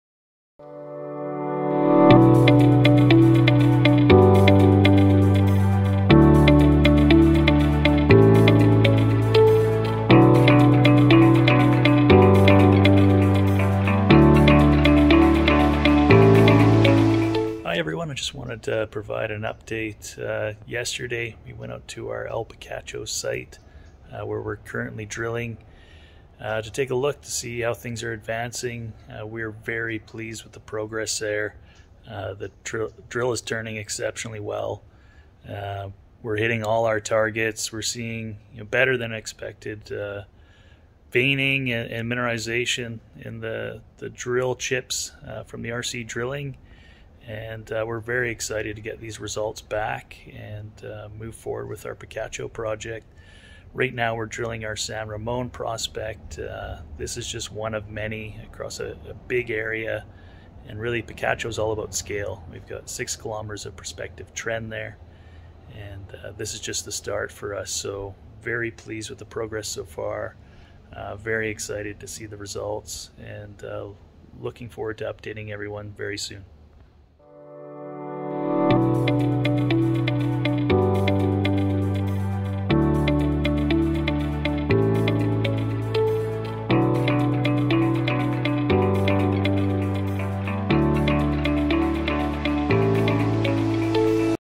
Tocvan Ventures - A Message From Our CEO, Brodie Sutherland
In this video, Tocvan Ventures CEO Brodie Sutherland discusses the ongoing maiden drill program at the El Picacho Project in Sonora, Mexico.

About the El Picacho Property

The El Picacho Gold-Silver property is interpreted as an orogenic gold system within the regional Caborca Orogenic Gold Belt known for producing gold mines that include La Herradura (10 Moz Au) and San Francisco (3 Moz Au). The project is 140 kilometers north of Hermosillo and only 18 kilometers southwest of the producing San Francisco Mine. The Project totals 24 square kilometers. Five primary zones of mineralization have been identified across the property totalling over six kilometers of prospective trends. Surface sampling and historic workings have identified high-grade gold and silver values. The project has only seen widely spaced reconnaissance drilling with no follow-up. Tocvan believes this provide an excellent opportunity for discovery of a muti-million ounce district.

All drilling completed to date has intersected mineralization, alteration and quartz veining that coincides with zones typically elevated in gold and silver. All samples will be assayed promptly. Results will be disseminated after passing QA/QC protocols.

The technical information in this video has been approved by Brodie Sutherland, P.Geo. and CEO of Tocvan Ventures. Mr. Sutherland is a Qualified Person as defined by National Instrument 43-101.